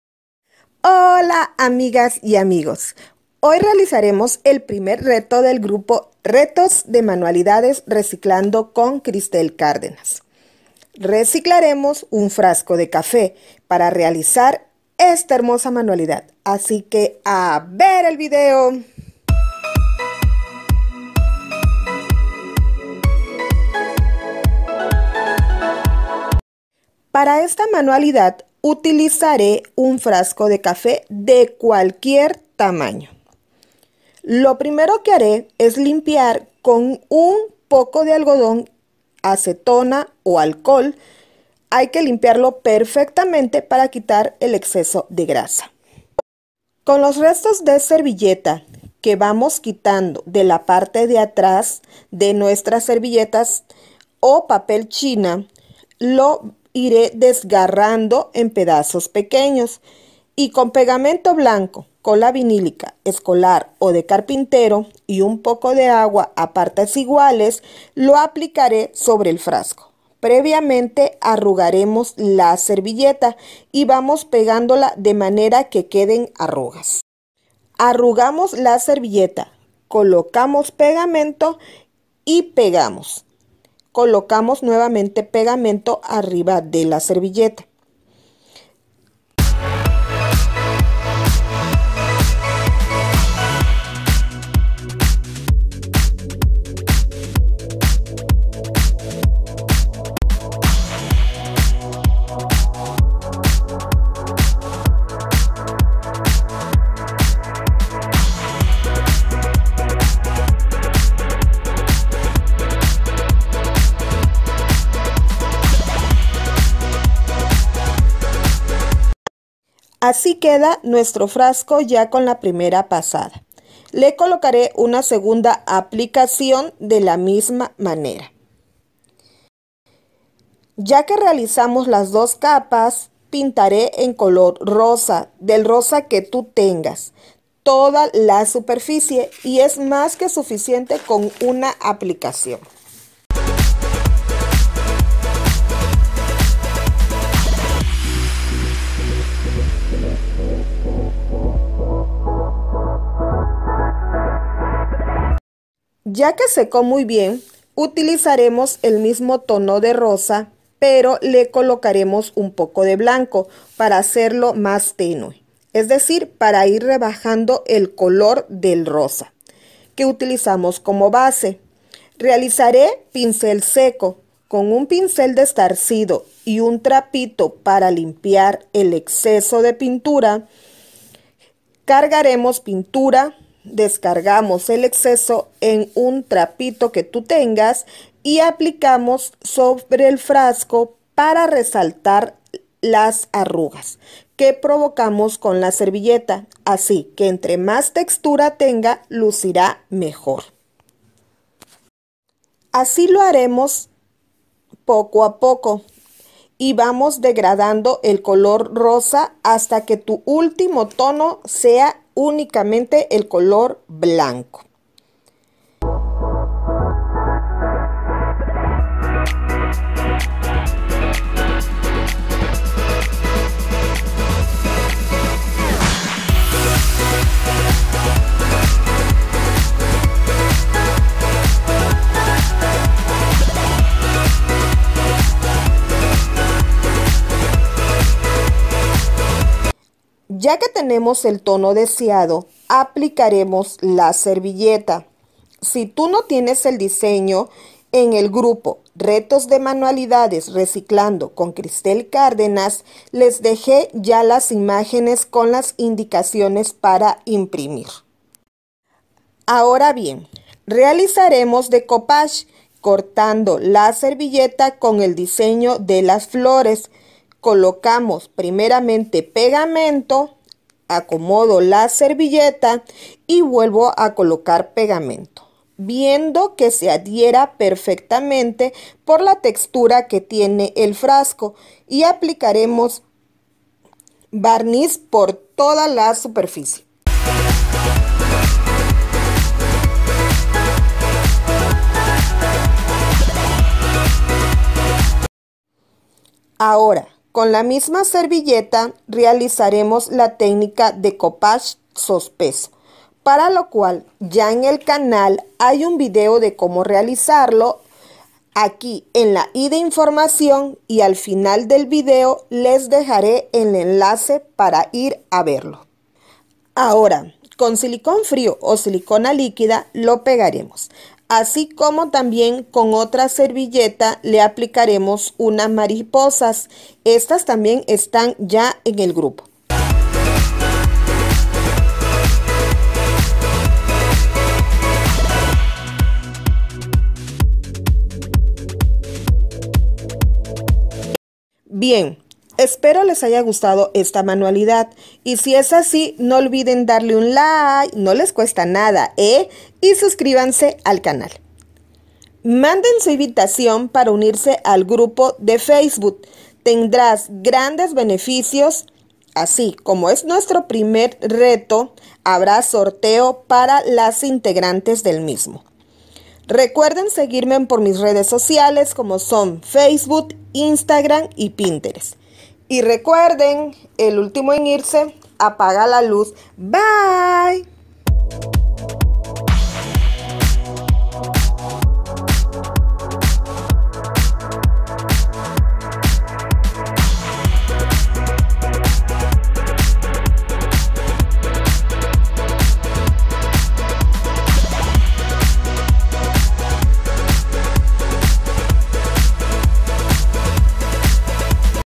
Reciclando frasco de café con decoupage sospeso RETO 1
Amigas y amigos, en este video tutorial verán como recicle un frasco de café, utilizando decoupage sospeso y reciclando las dos capas posteriores de nuestra servilleta, como primer RETO del grupo RETOS DE MANUALIDADES RECICLANDO CON CRISTELL CARDENAS, así que manos a la obra.

Materiales:
frasco de café
pegamento blanco
servilletas alemanas
pintura acrilica blanca y rosa
pegamento para decoupage

Unete a mi grupo: RETOS, de manualidades reciclando con Cristell Cárdenas.